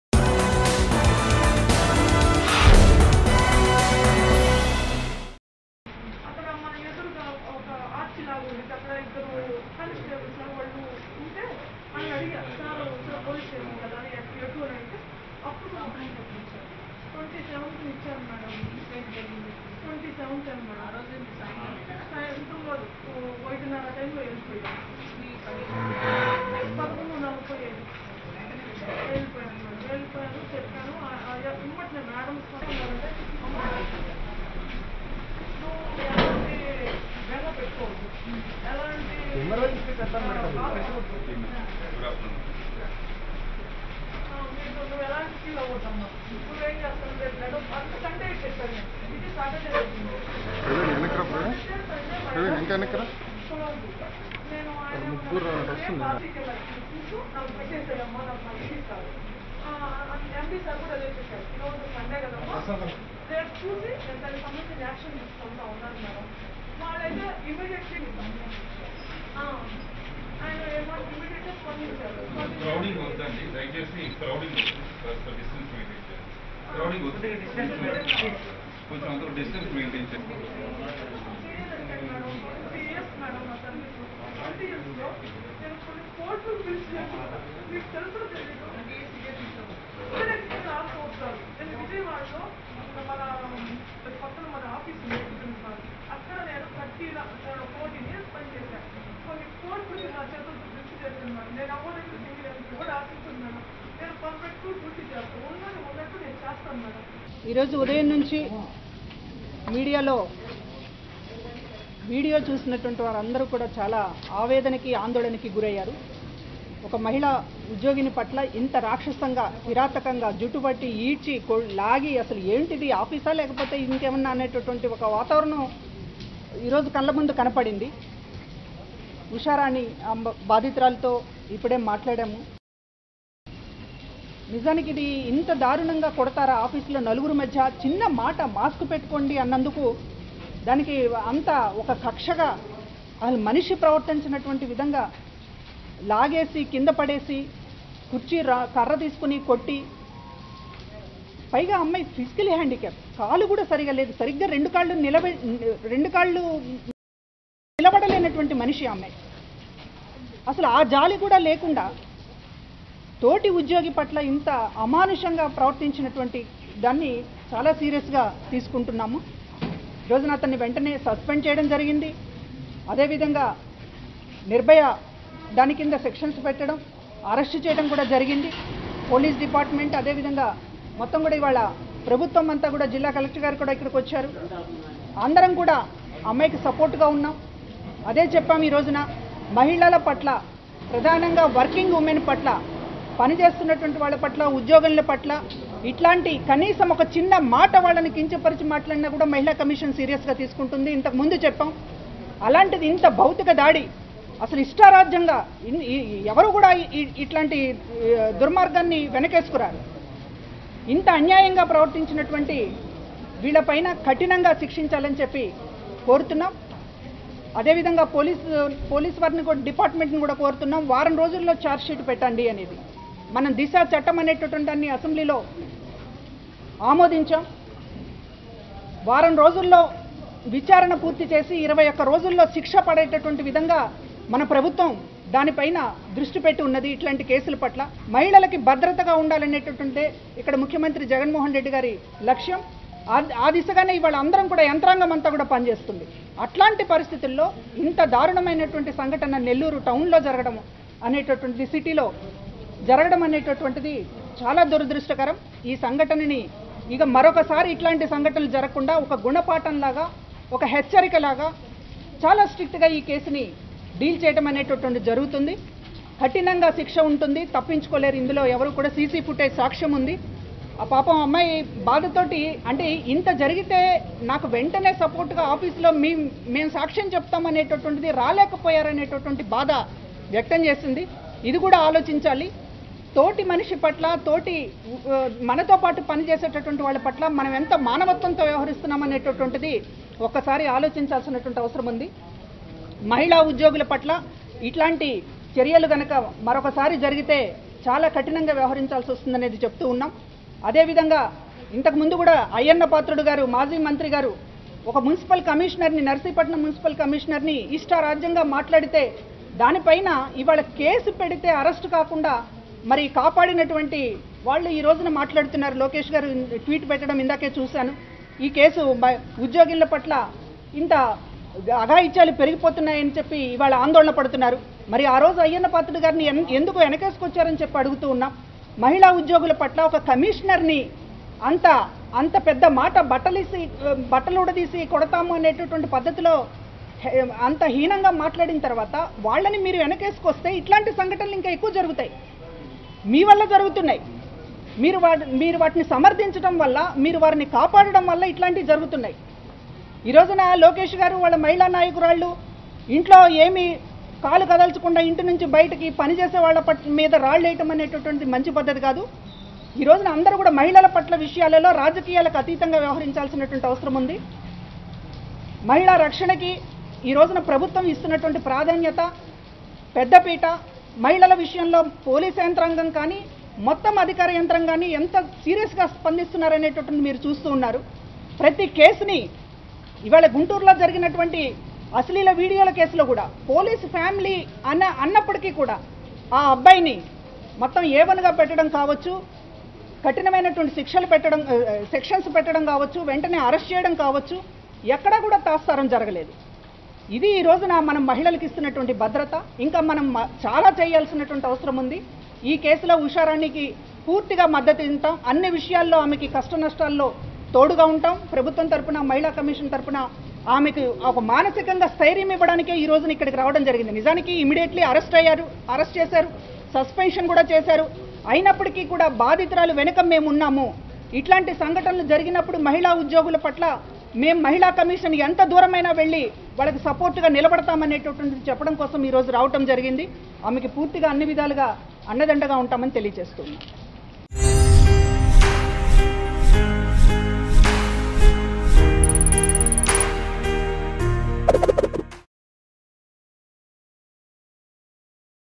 AP Women Commission Chairperson Vasireddy Padma Consoled Woman Victim | AP Latest News | Mango News
AP Women Commission Chairperson Vasireddy Padma Consoled Woman Victim.